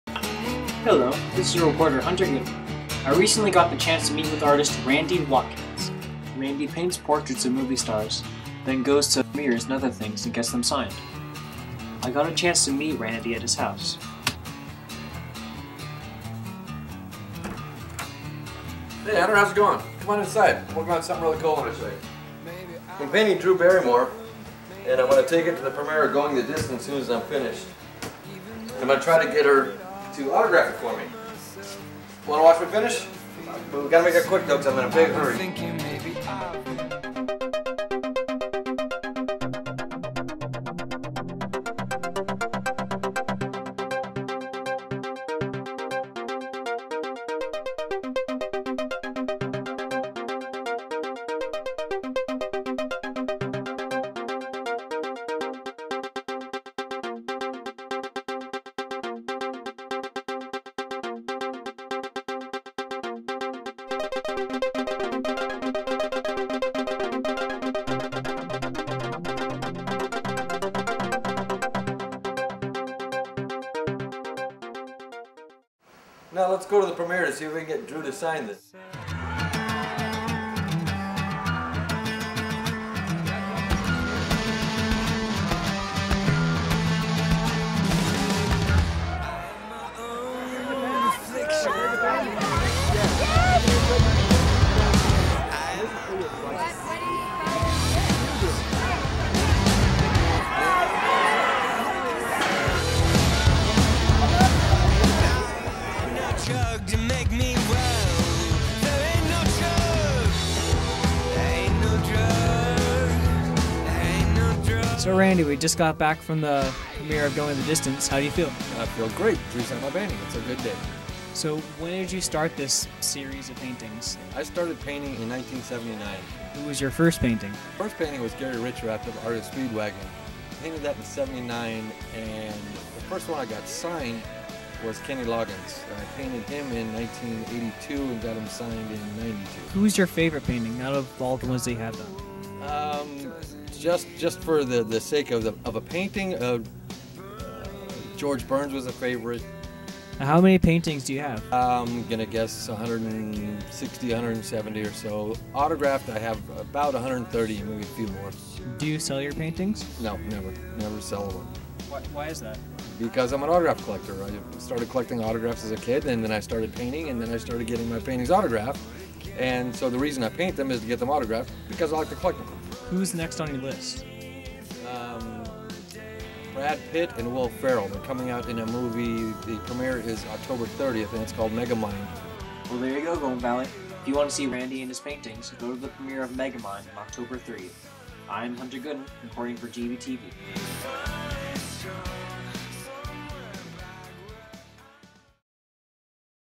Hunter's version of Going The Distance with Drew Barrymore signing at the premiere
Hunter shot, mixed and edited this version of Drew Barrymore at the premiere for Going The Distance.